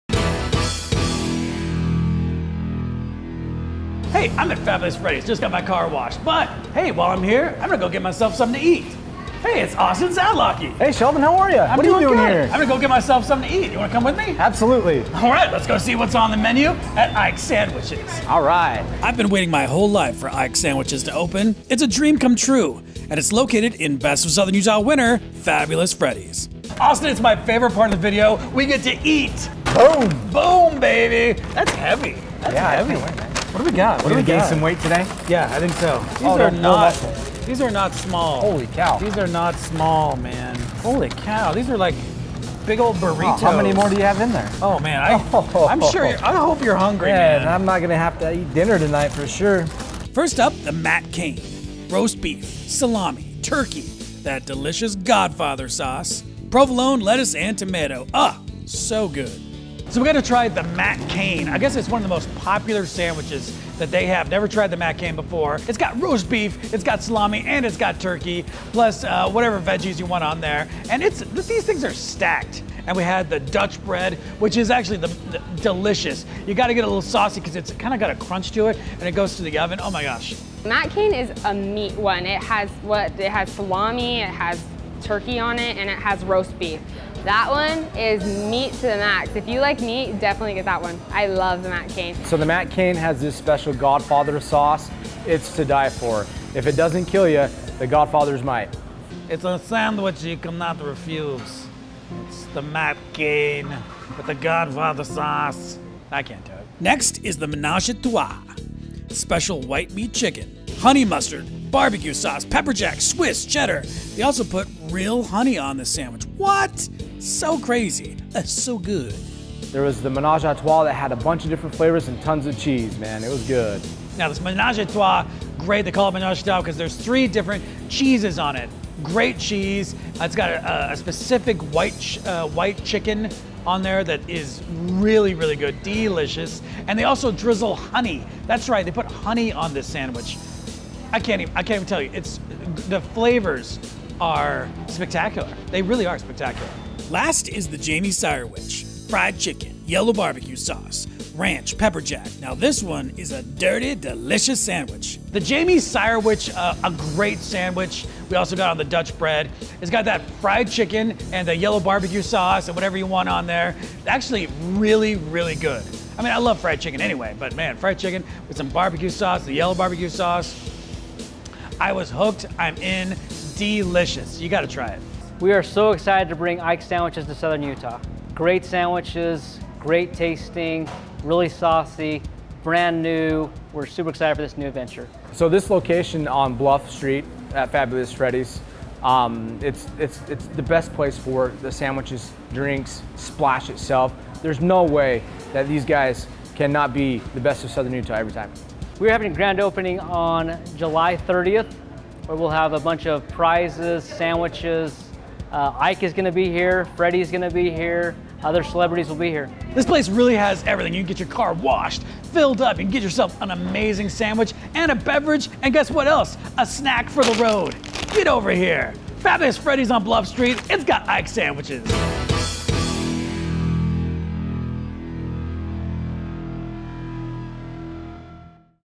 What’s on the Menu: Ike’s Love & Sandwiches
ST. GEORGE — “What’s on the Menu” host Sheldon Demke loves trying out new places to eat, and one sandwich shop is so new that it’s actually opening today: none other than Ike’s Love & Sandwiches.
It’s a dream come true, and it’s located inside a great business that locals know, love and voted the finest full-service car wash during the 2022 Best of Southern Utah contest: Fabulous Freddy’s on Bluff Street. Accompanied by Austin Szatlocky with Ken Garff St. George Ford, Demke dove headfirst into a stack of mind-blowing sandwiches.